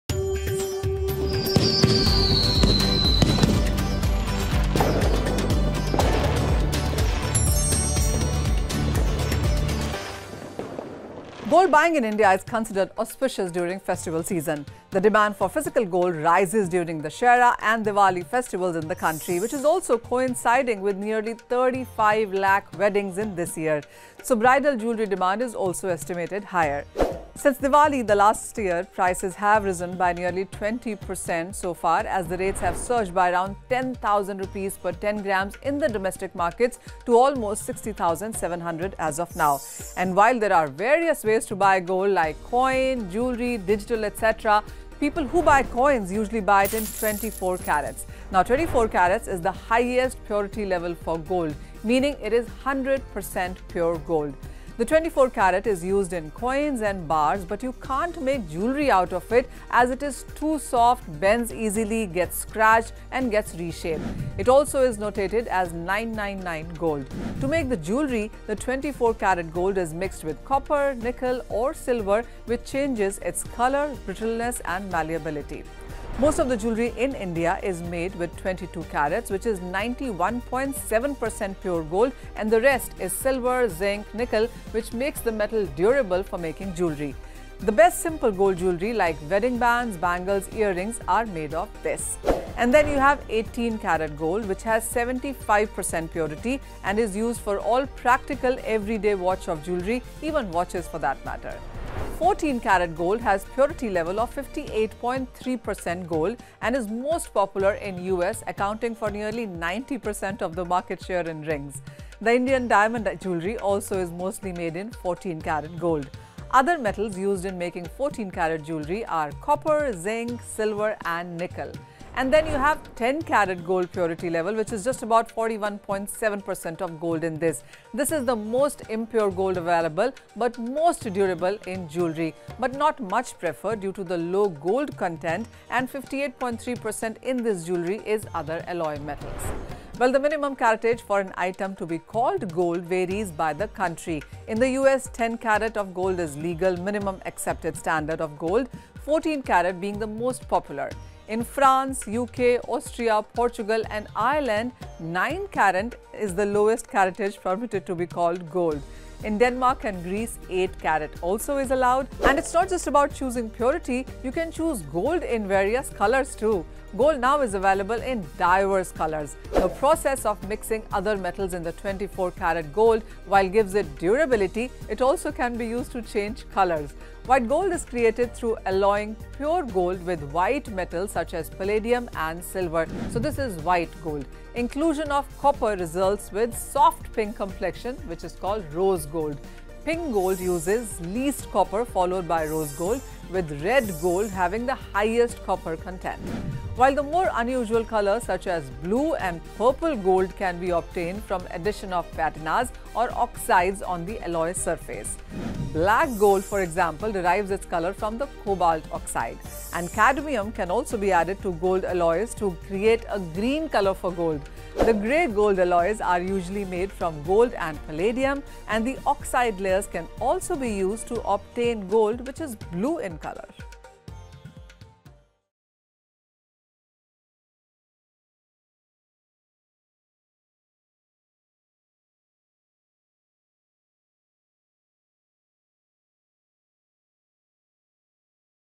Colours & Caratage Of Gold: What Metals Are Mixed To Get Pink, Green, Blue & Black Gold
Shopping for gold this festive season? Well, gold doesn’t just need to be the “yellow” metal - there is rose gold & white gold, which many of us are aware of. But did you know there is also green gold, blue gold and even black gold? Watch to find out more!